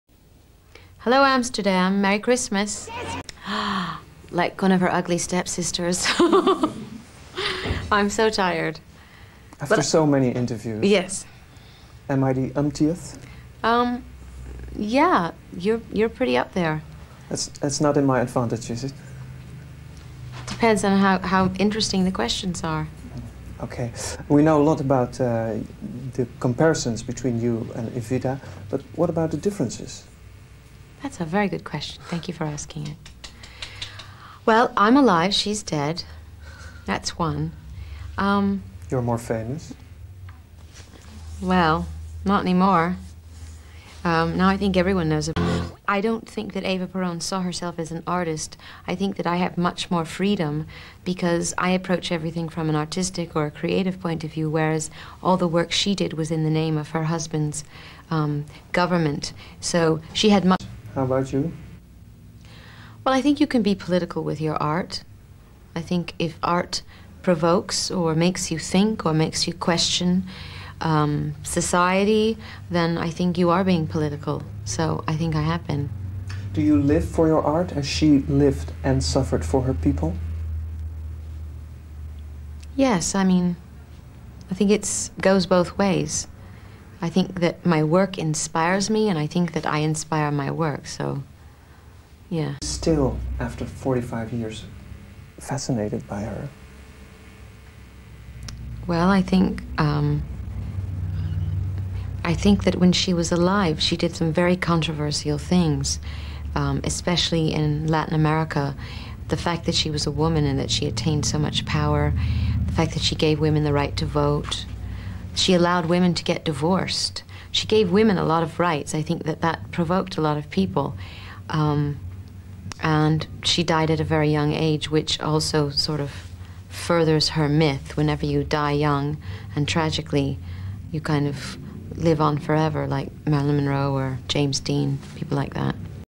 Madonna Interview Footage Video Hollywood Star Movie Star Music Camera Show Cinematography Channel
Madonna Interview 🎙️
Interview Recovered Video HD 1996
Cinematography ⭐ Hollywood

Madonna Louise(a) Ciccone is an American 🇺🇸 singer-songwriter, pop singer, dancer, record producer,
actress🎦, director, and children's writer.  The most commercially successful performer in history according to the Guinness Book of Records.
Date and place of birth: August 16, 1958 (age 63), Bay City, Michigan, USA